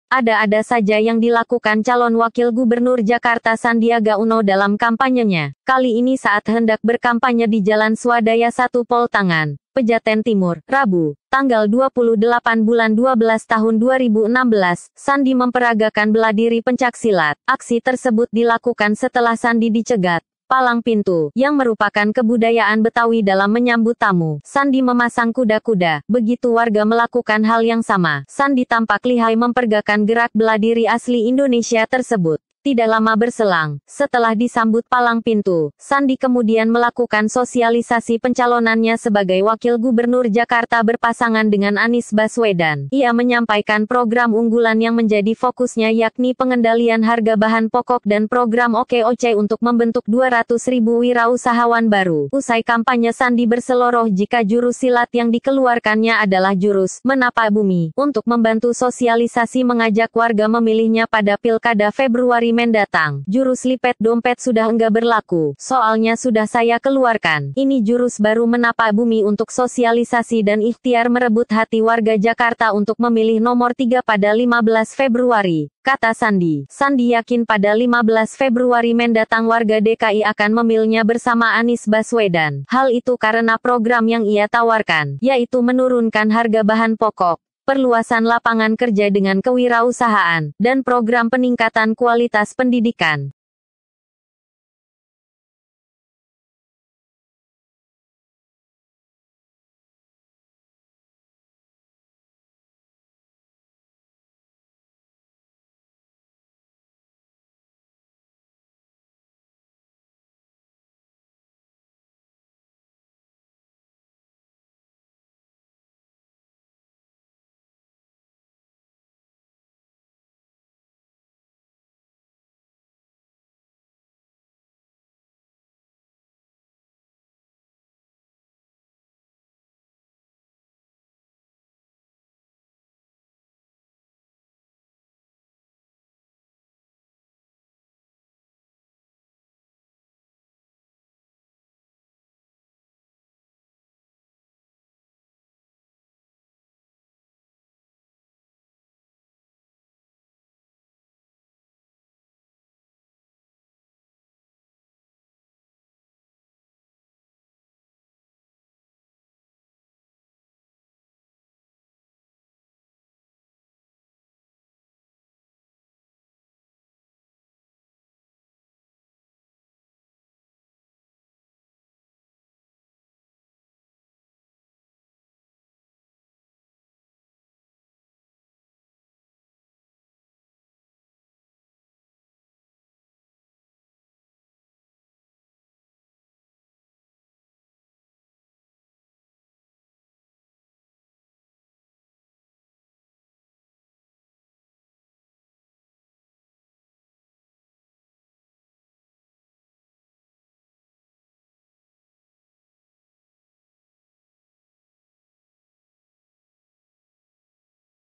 "Dihadang" Palang Pintu ,Sandiaga Keluarkan Jurus Tapak Bumi
Sandiaga memperagakan jurus silat asli indonesia saat berkampanye